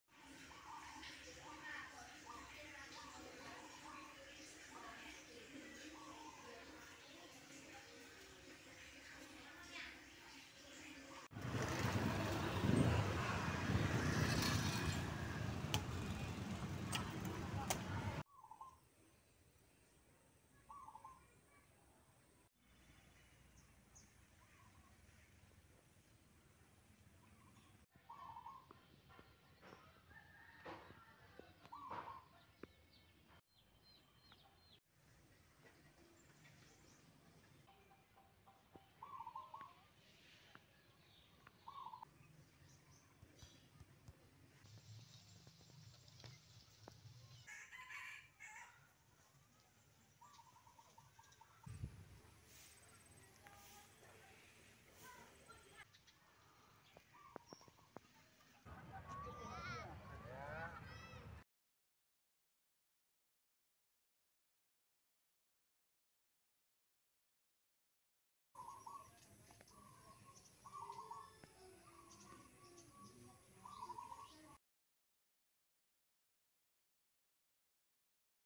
cinematic video II Belajar cinema shot ramdom II tanpa musik karna takut musik Copyright.II
belajar mengambil beberapa footage untuk di gabungkan menjadi sebuah video, saya tidak memasukkan musik saya takut mempengaruhi chanel saya. saya belum tahu caranya supaya bisa memaksimalkan video saya dan pengaruh lagu yang copyright